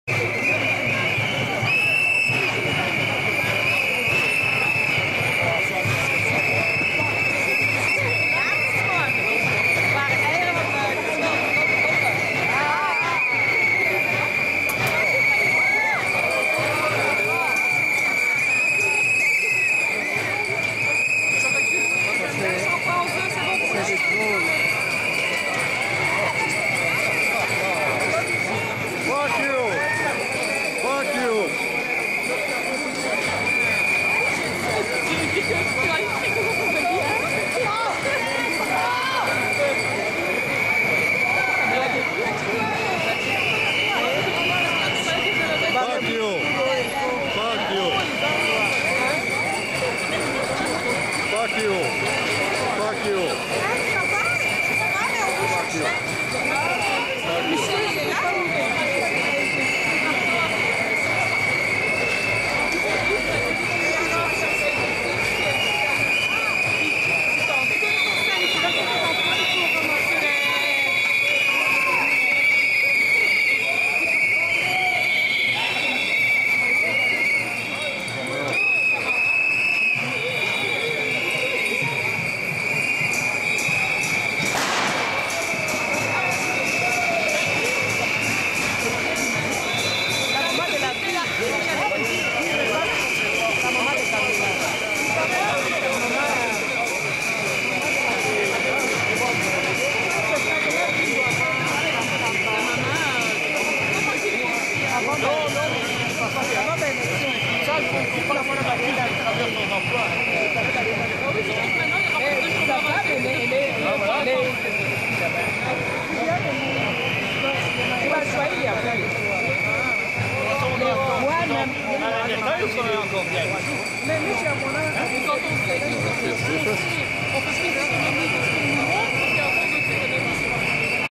UNION RALLY BRUSSELS DEC 2 2012 -14- egg throwing on liberal party by green unionists
80.000 people protested against austerity measures